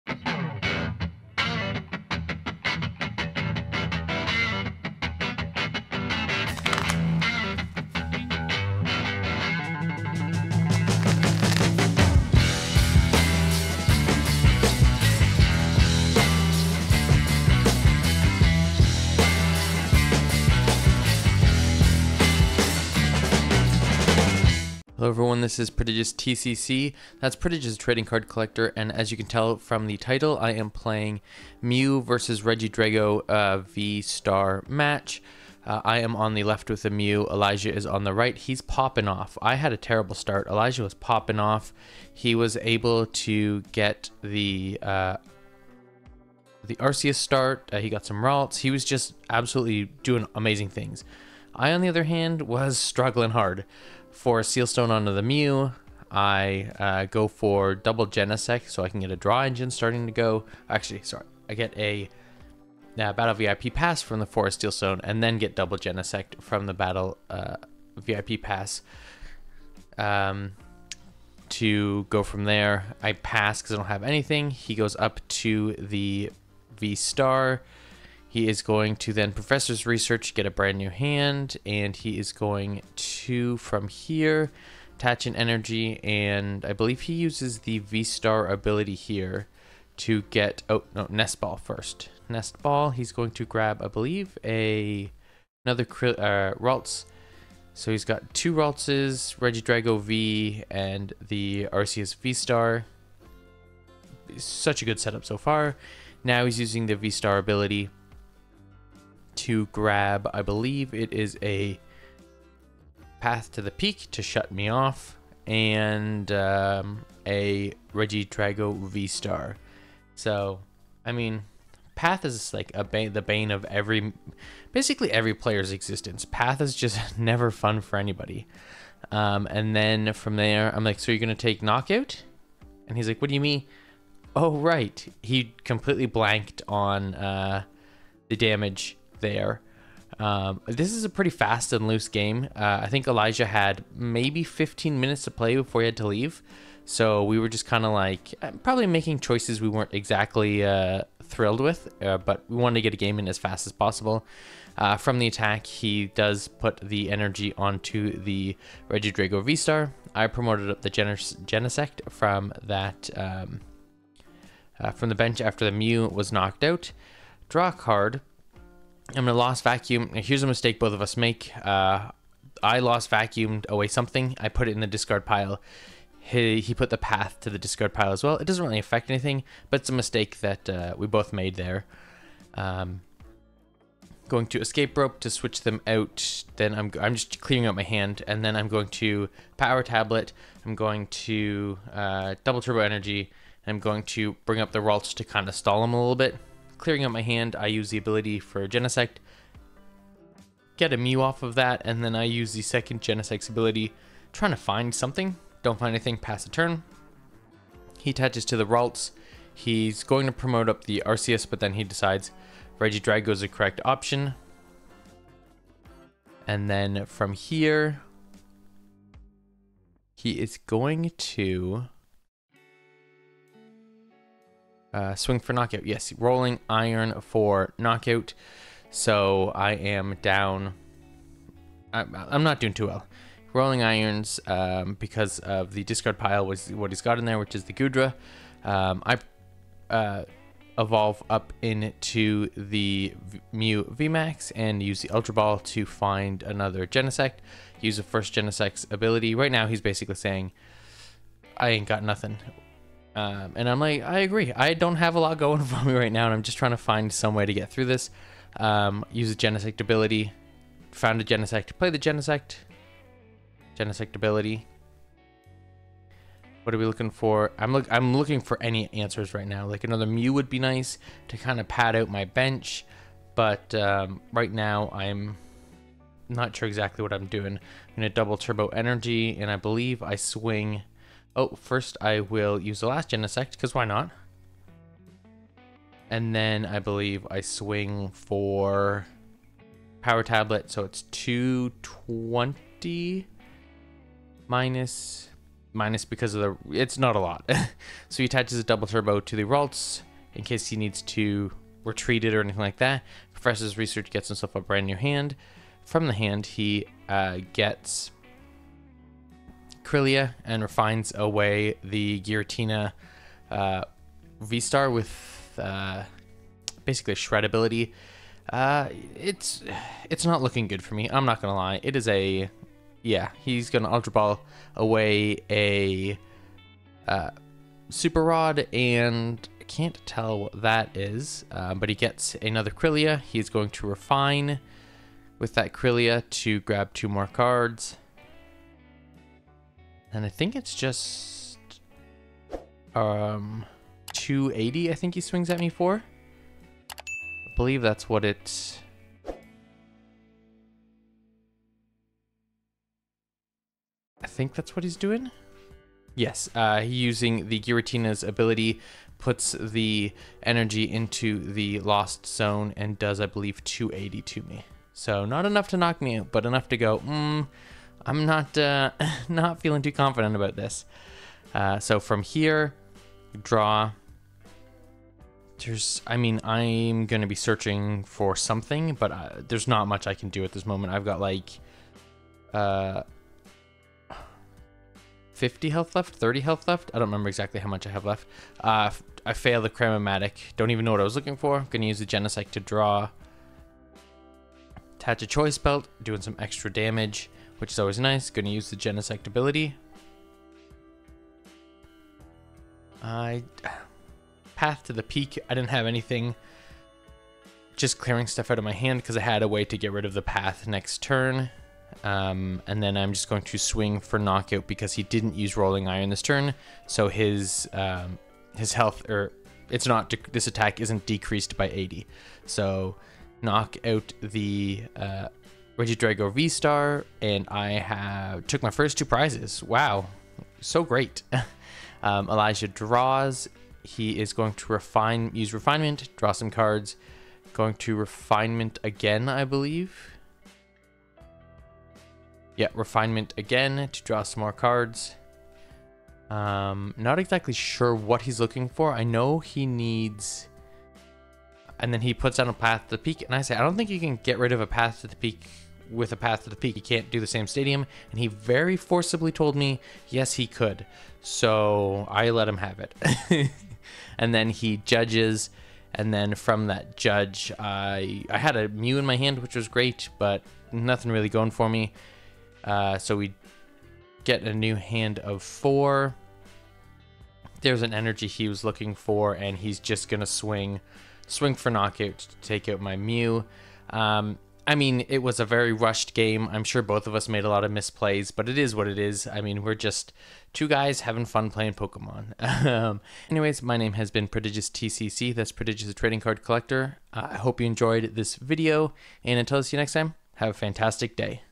Mew Vmax VS Regidrago Vstar || Sleeve Up & Smack Down Ep. 12
I'm back playing Pokémon and today I am using my Mew Deck and am playing in a fast paced match against Elijah's Regidrago Deck. Who will come out on top in this match up.